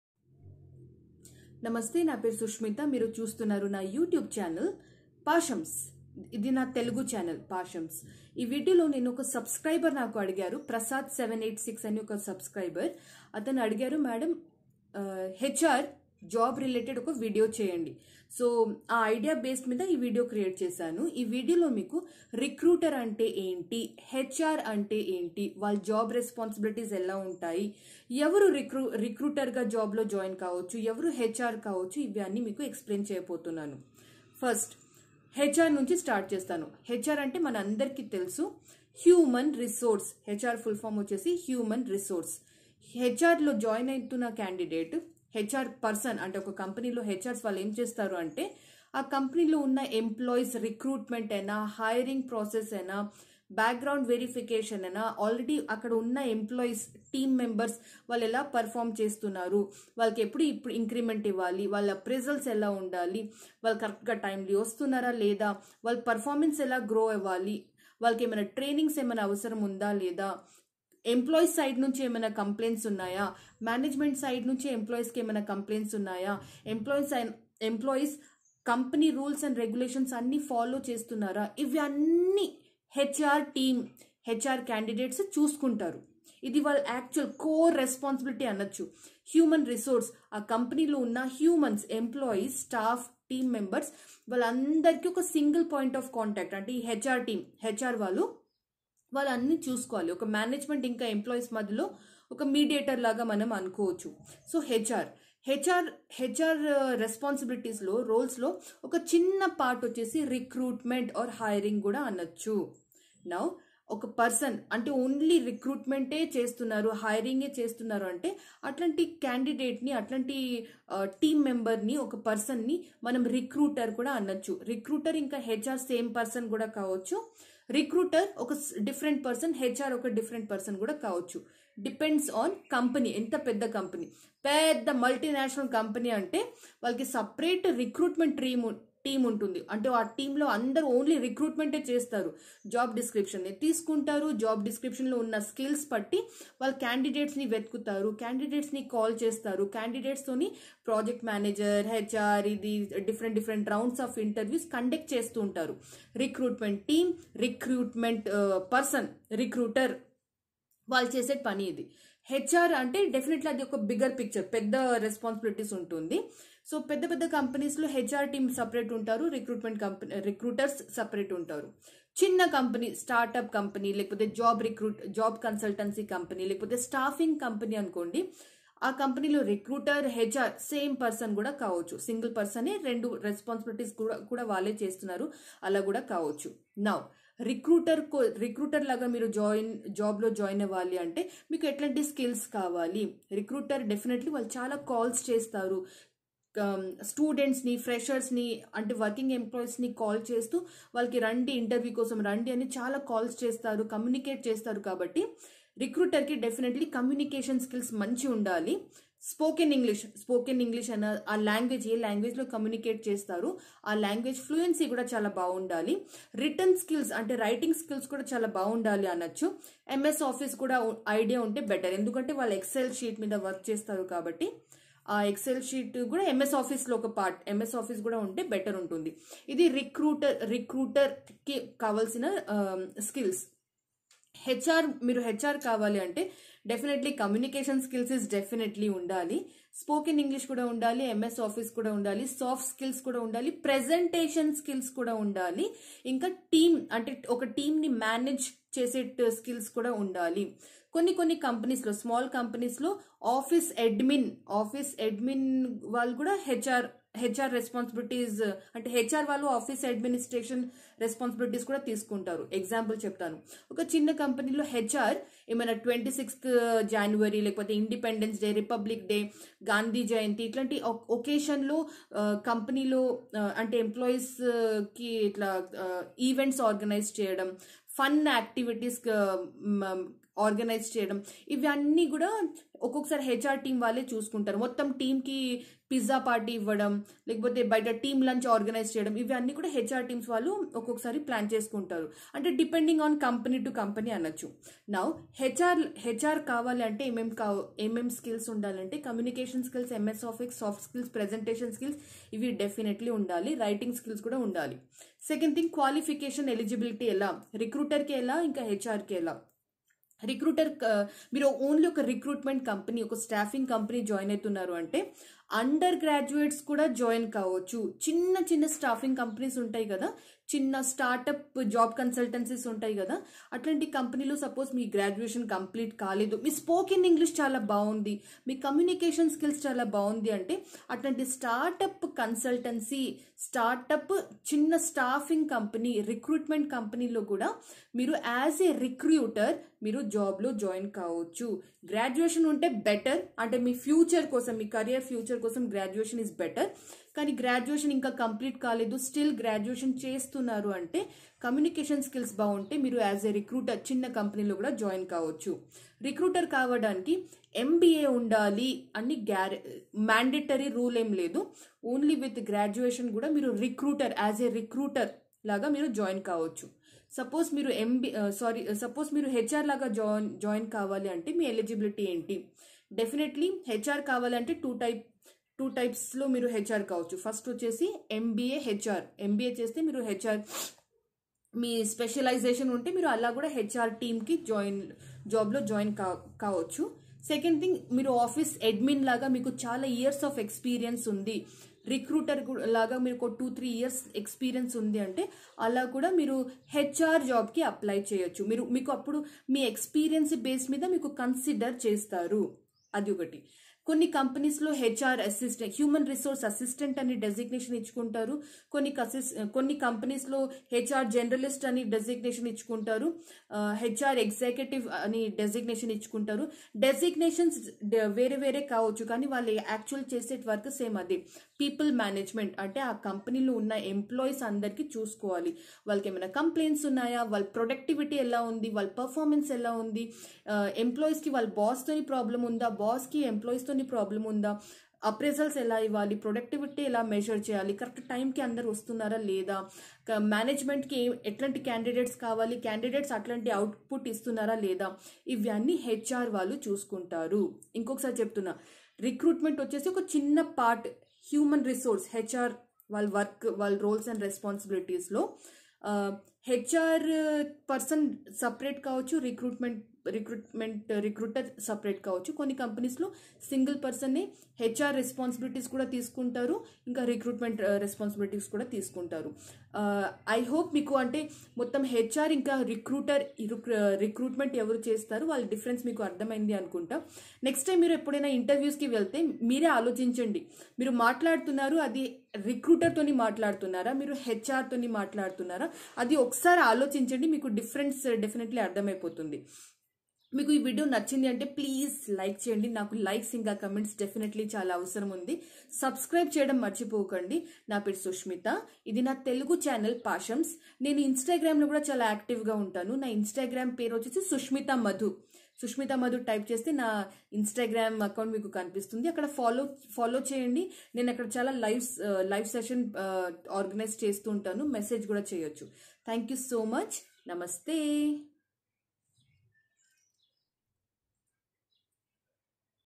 What is HR job role & responsibilities - Explained in Telugu
In this video I have explained about HR job roles and responsibilities. what exactly they work in office what does a recruiter do, what is recruiter job roles and responsibilities. comment below your inputs.